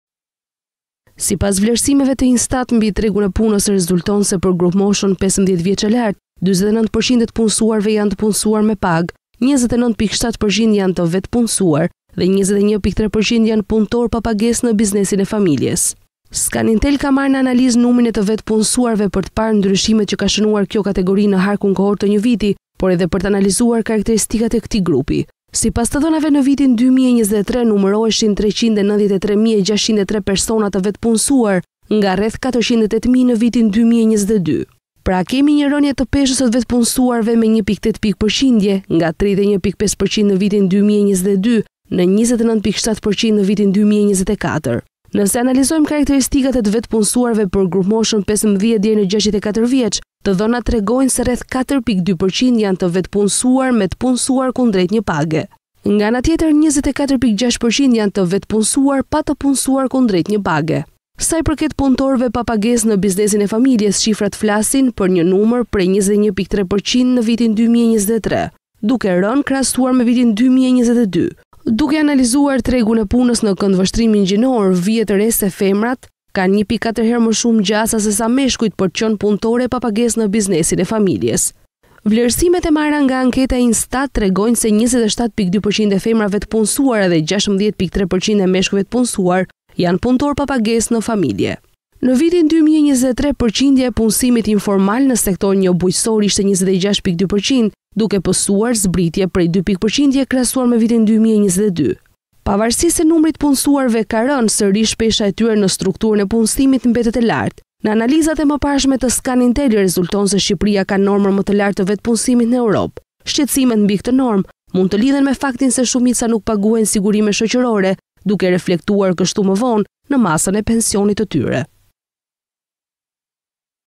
Bie numri i të vetëpunësuarve në vitin 2023 – Zënë 29.7% të peshës, nga 31.5% që ishte...
Sipas vlerësimeve të INSTAT mbi tregun e punës, rezulton se për grupmoshën 15 vjeç e lart, 49.0% e të punësuarve janë të punësuar me pagë, 29.7% janë të vetë-punësuar dhe 21.3% janë punëtorë pa pagesë në biznesin e familjes.